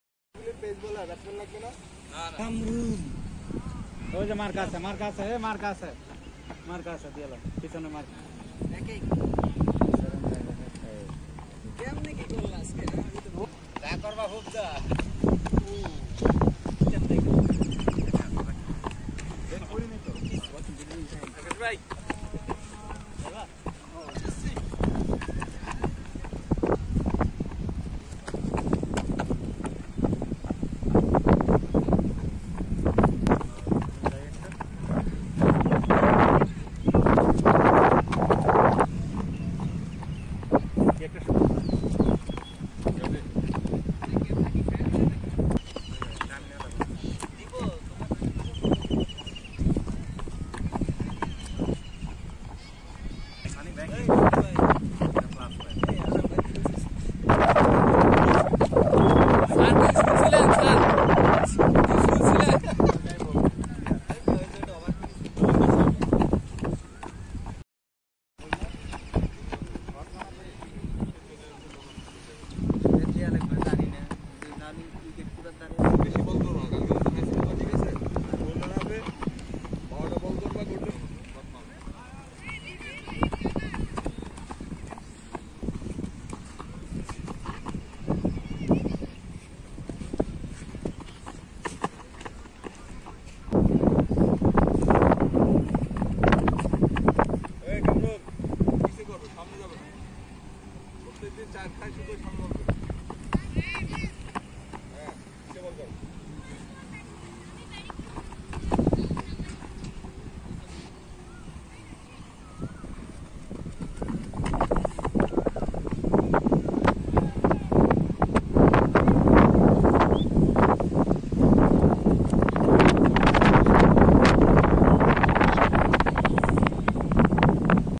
গ্রুপ ৬ এর সেন্টার উইকেটে পেজ বলিং অনুশীলন কোচ সৈয়দ রাসেল
senior coach Mehedi Hasan sir
 coach Mohammad Ashraful Ziko sir
coach Mostakim Mushy sir
Coach Taj sir
Coach Sahin sir
Trainer Bappy sir
phyjio Uzzal Zahurul